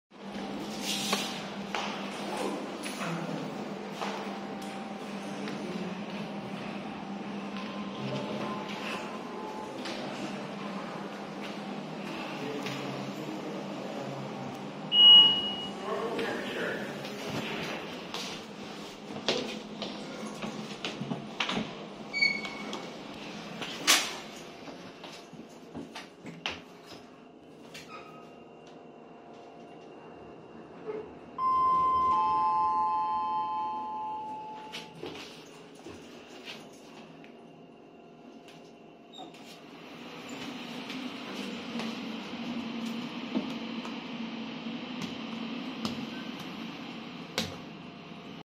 (FLOOR LOCKED) A ThyssenKrupp Bomba Lif at FOX Life DPulze Cyberjaya, Sepang, Selangor, Malaysia
What the Hell I almost got trapped and again Indicator Busted